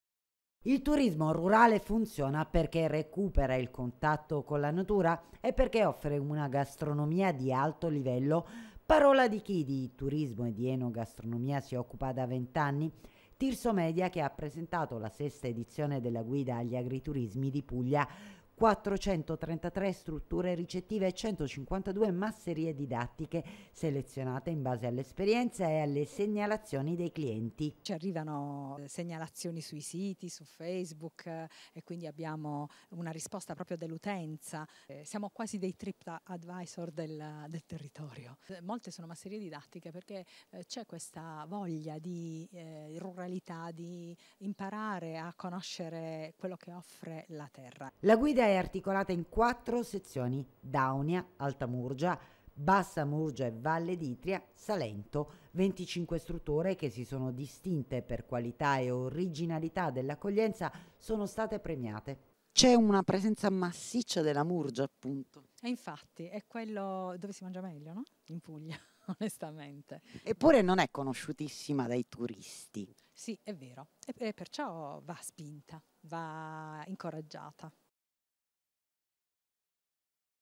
BARI GUIDA AGRITURISMO
PRESENTAZIONE A BARI DEI MIGLIORI AGRITURISMI DI PUGLIA RACCOLTI IN UNA GUIDA DEDICATA AL TURISMO RURALE.